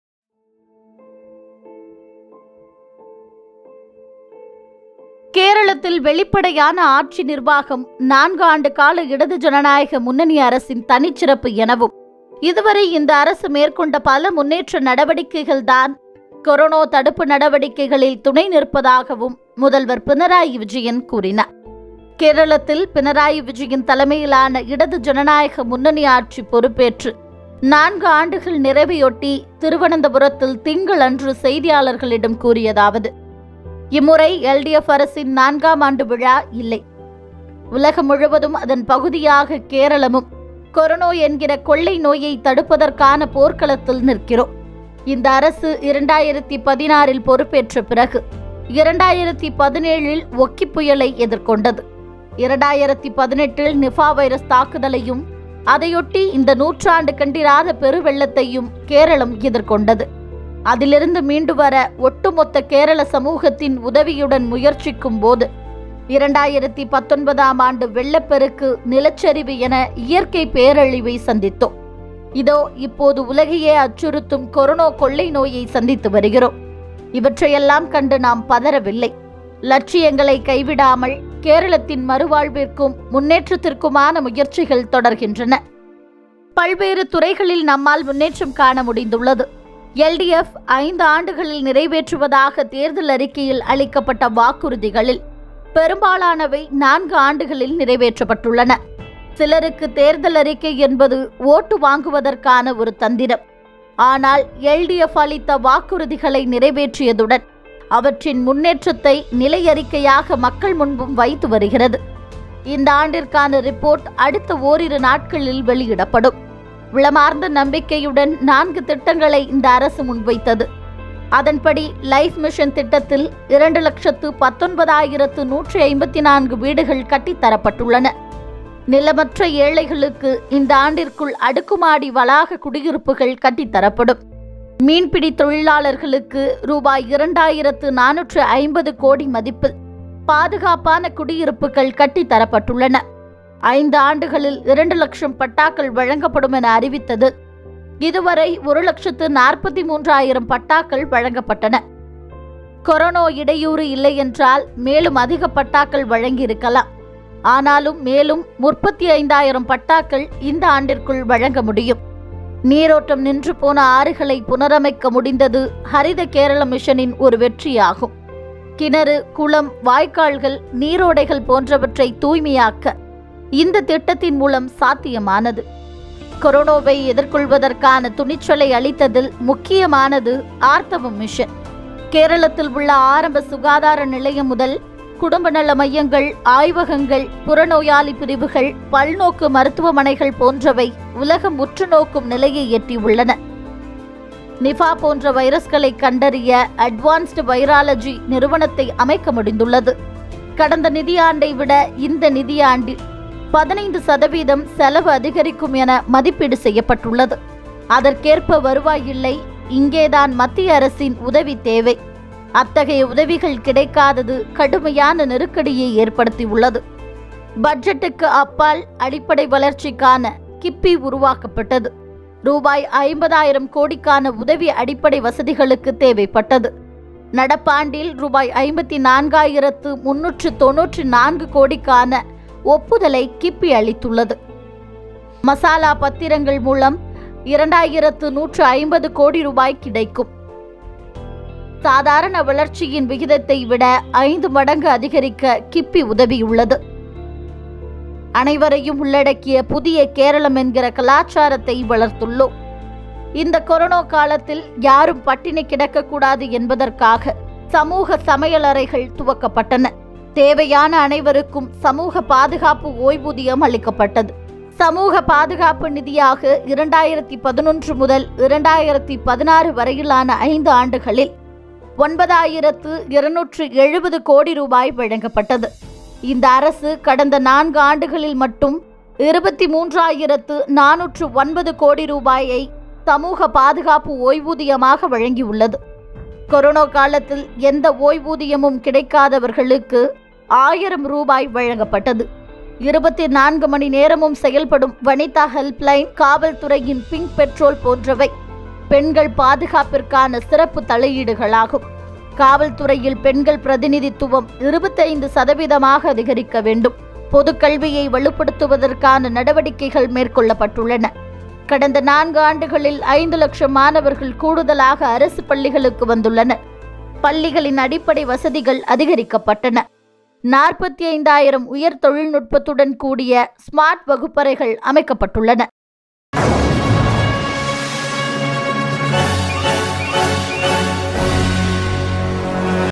இடதுசாரி அரசின் வெளிப்படையான நிர்வாகத்தின் 4 ஆண்டுகள் | பினராயி விஜயன்
ஒக்கிப்புயல், நிபா வைரஸ், நூற்றாண்டுப் பெருவெள்ளம் தற்போது கொரோனா என இயற்கைப் பேரிடர்களை வெளிப்படைத் தன்மையுடன் எதிர்கொண்டு லட்சியப் பாதையில் முன்னேறும் தோழர் பினராயி விஜயன் தலைமையிலான கேரள இடது ஜனநாயக முன்னணி அரசின் 4 ஆண்டுகள்.